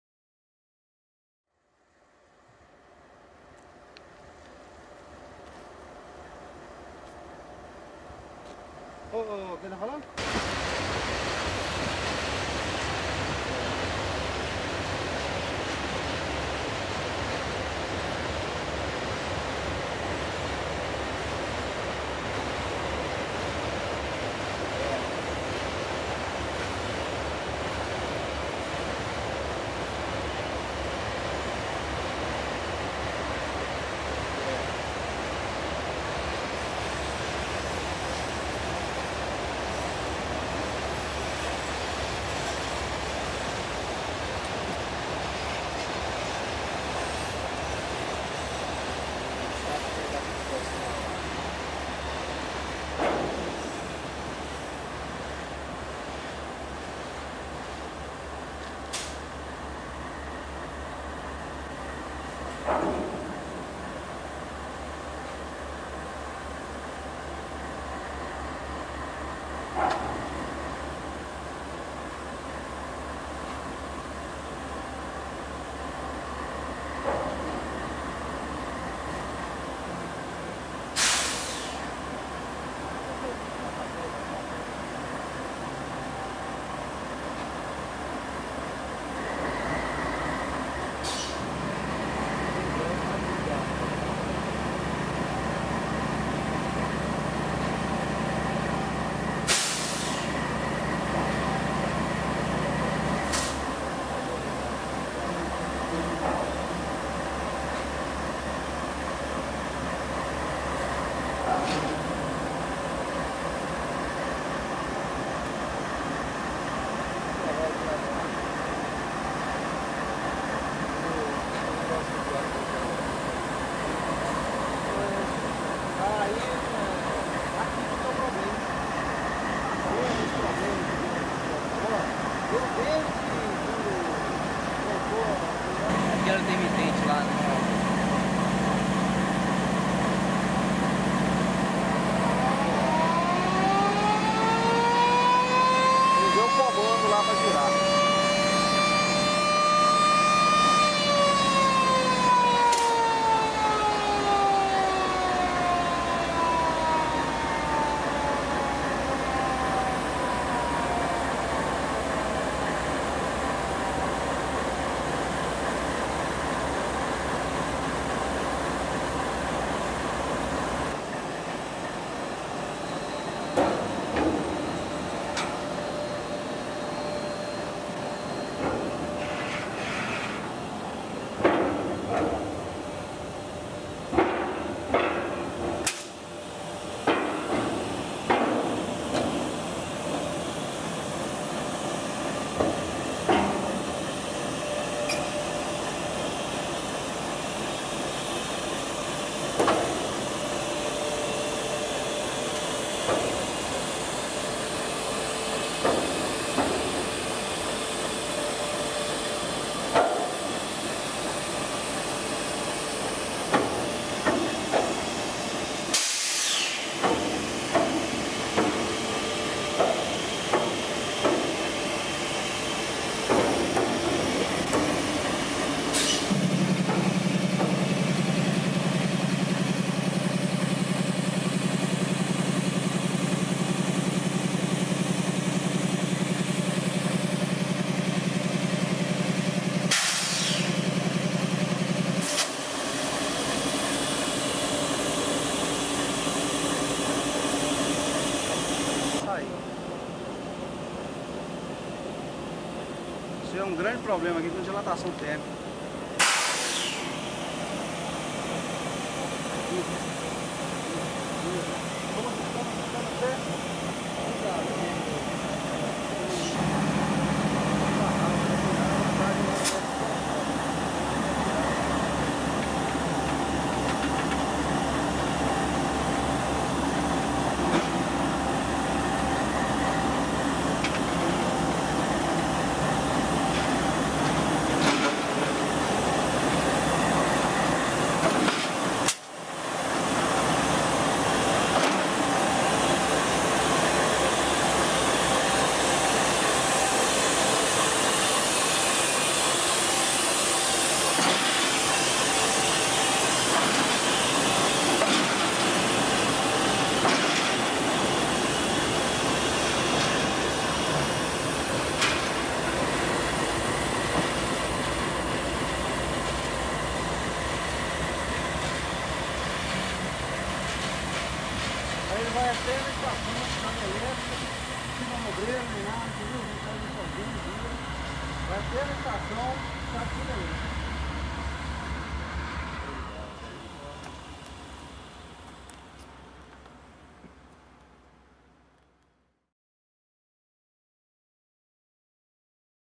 CVRD Ipatinga MG Dash BB no virador 2003
Locomotiva GE Dash 9 BB40-9W no virador de Ipatinga MG em 2003